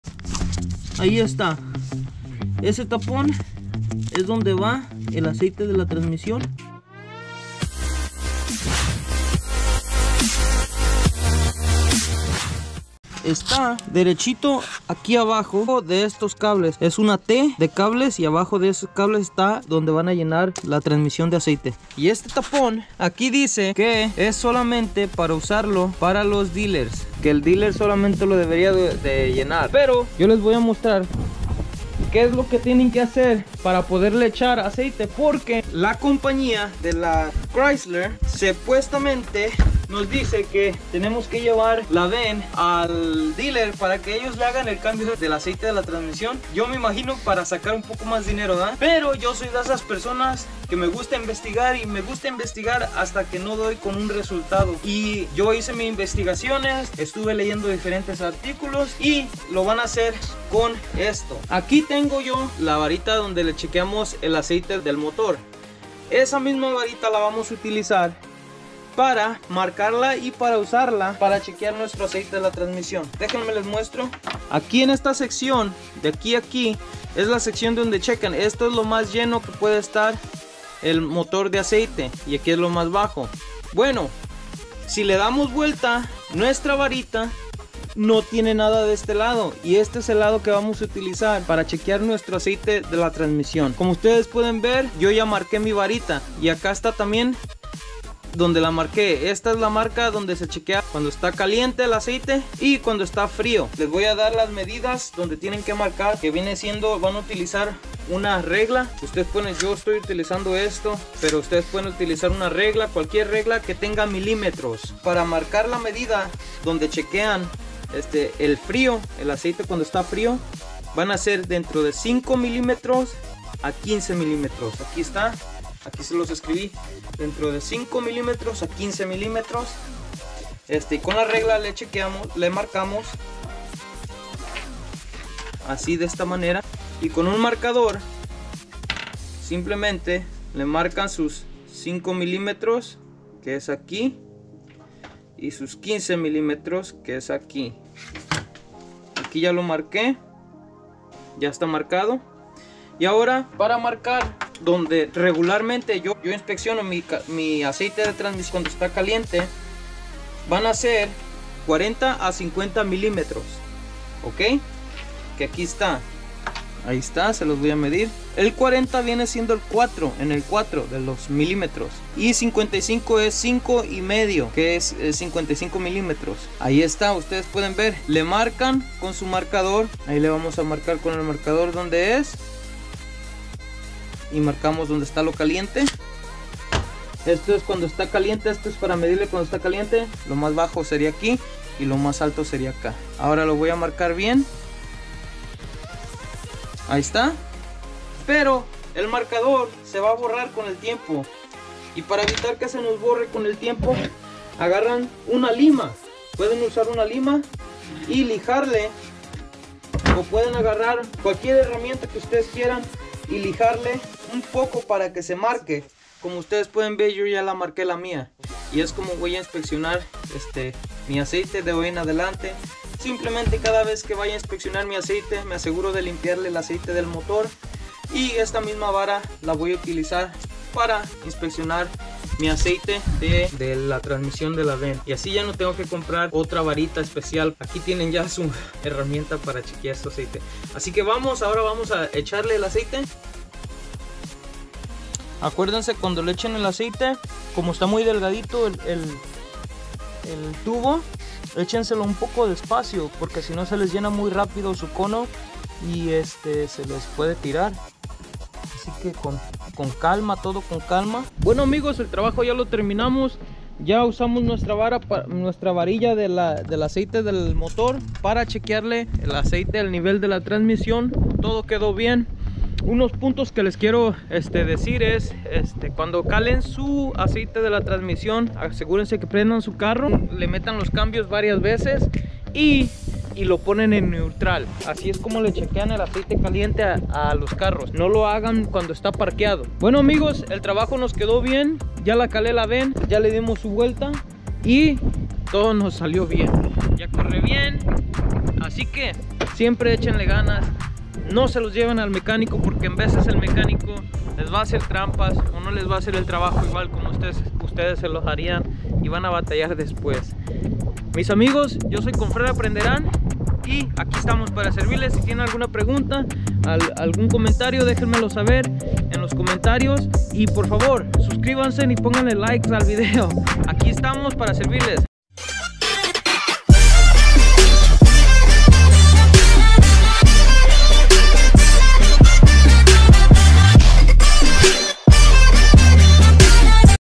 Chrysler Town & Country: Como ponerle aceite de transmission 2009 - 2014 (Parte 2)
En ésté video te muestro lo que tienes que hacer para ponerle aceite de transmisión a una Chrysler Town & Country ven. También te muestro lo que tienes que ser para medir tu aceite.